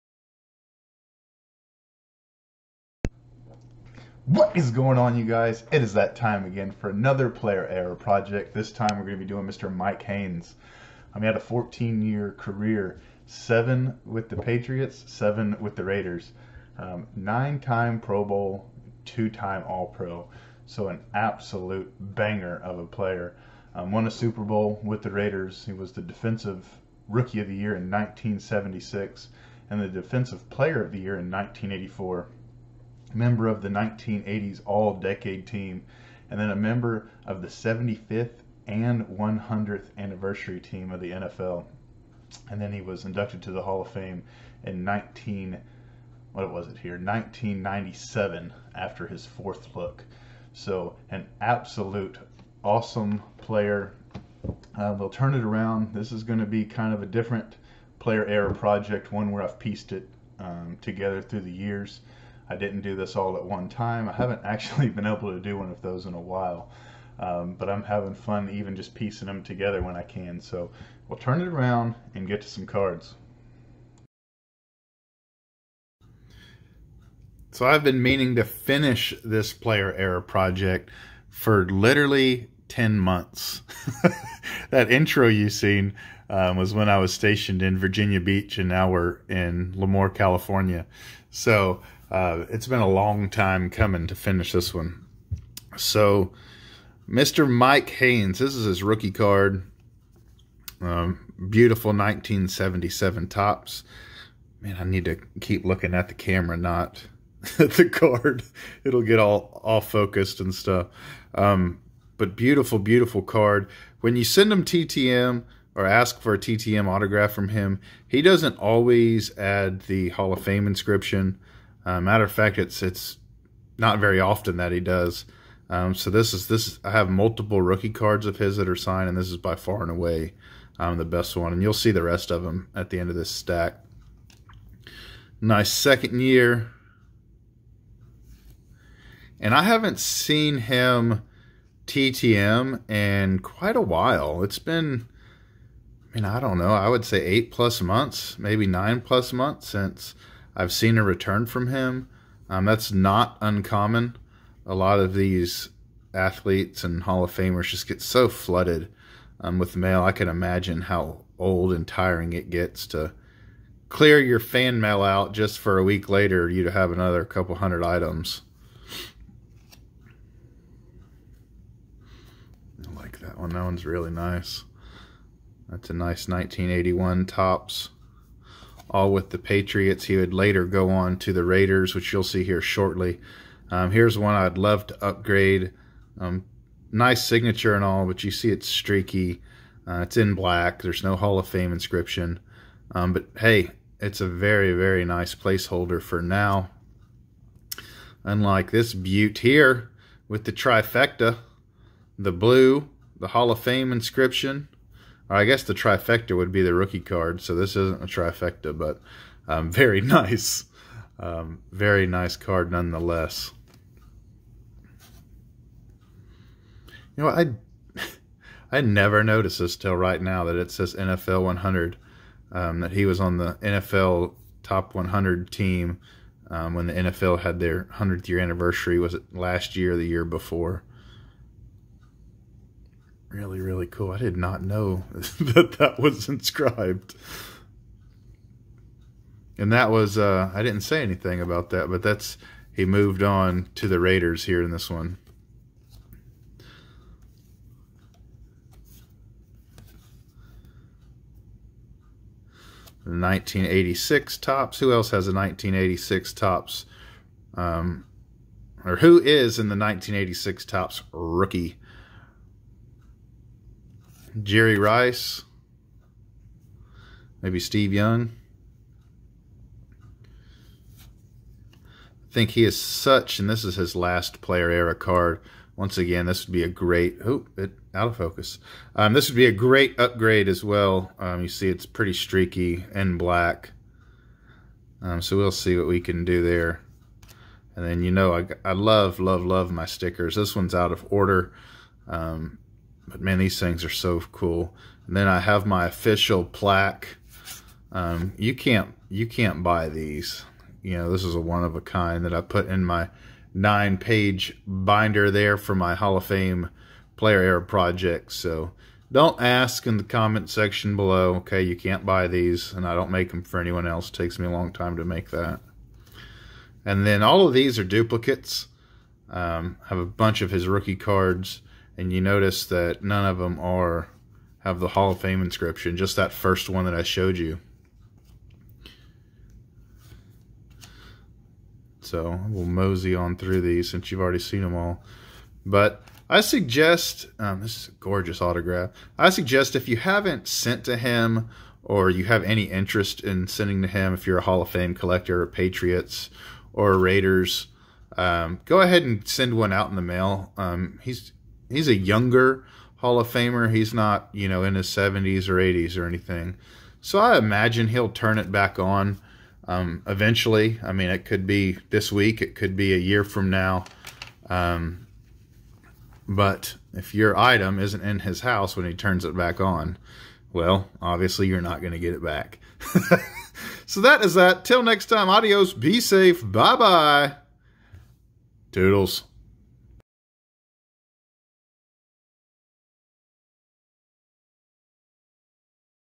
The Mike Haynes HOF TTM Autograph "Player Era Project"
Michael Haynes was a cornerback in the NFL for the New England Patriots and the Los Angeles Raiders. He used his speed, quickness and range to become both a premier defensive back and an outstanding punt return specialist. Haynes was inducted into the College Football Hall of Fame in 2000 and the Pro Football Hall of Fame in 1997.